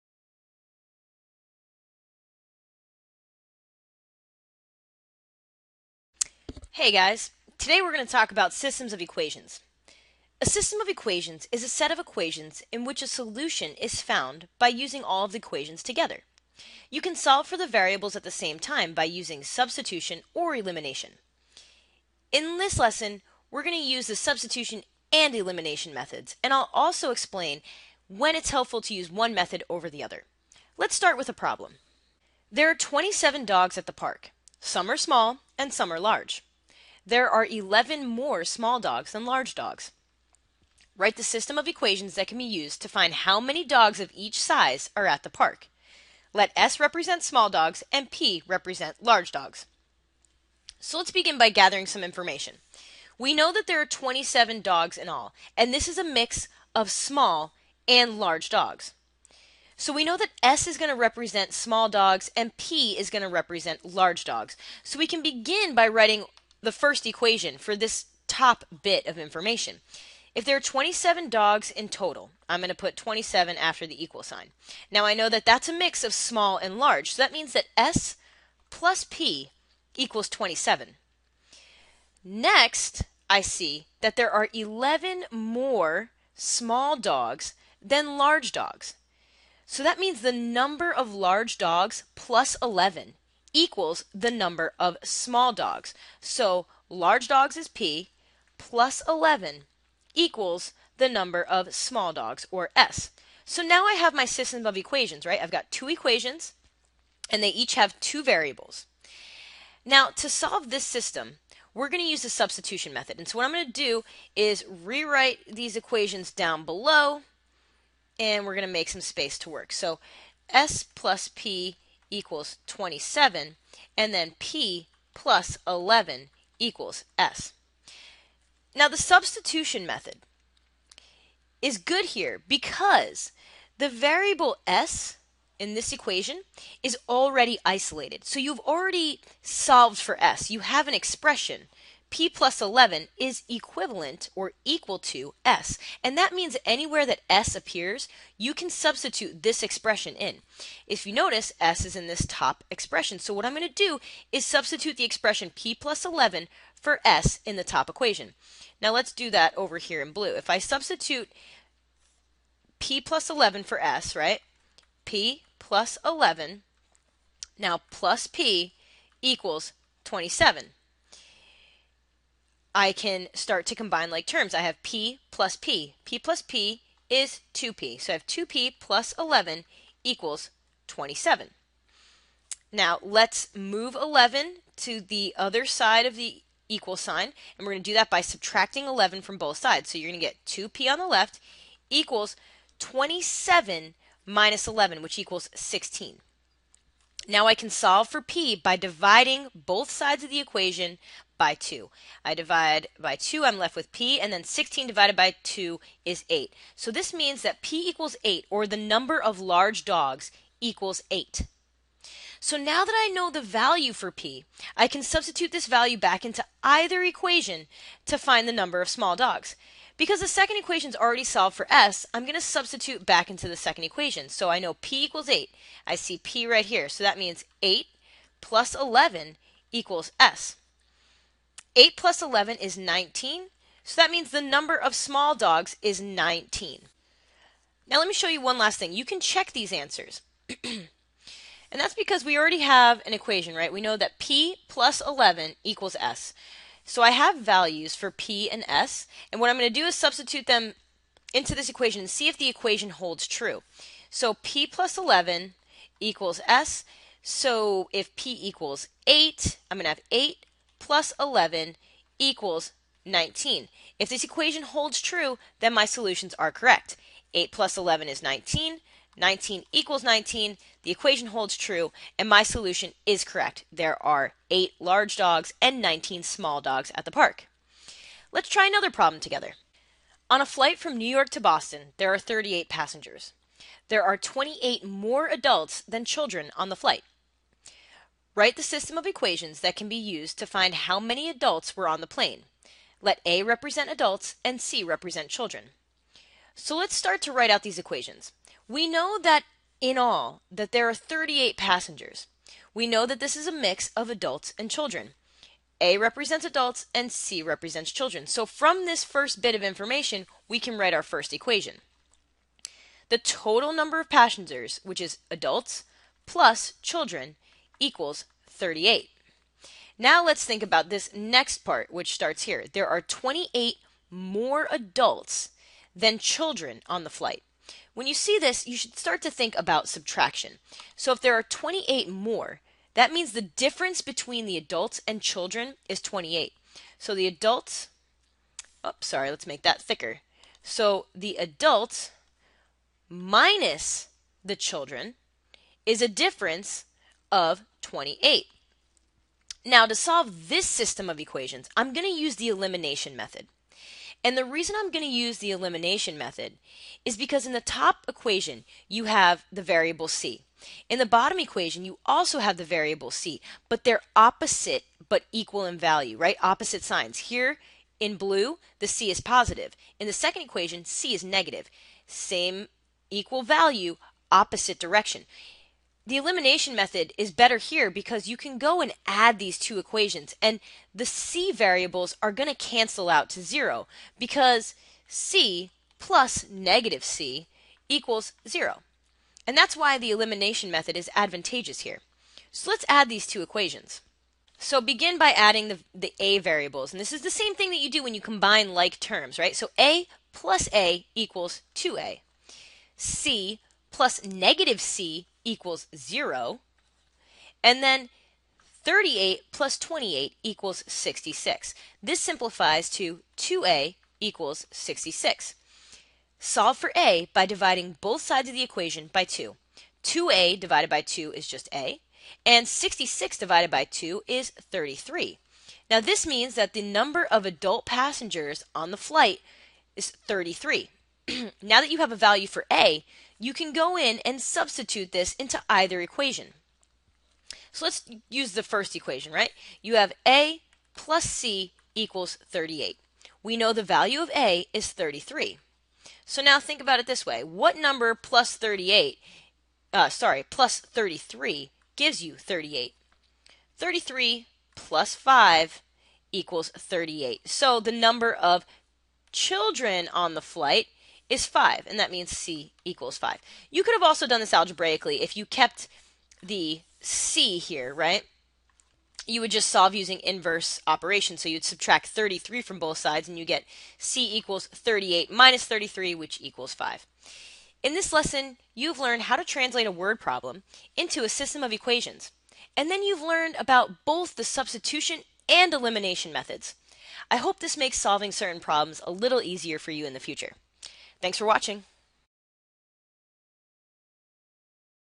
Solving Word Problems Involving Systems of Equations: 8.EE.8c
This video explains how to solve word problems involving systems of equations using substitution and elimination and describes when to use each method.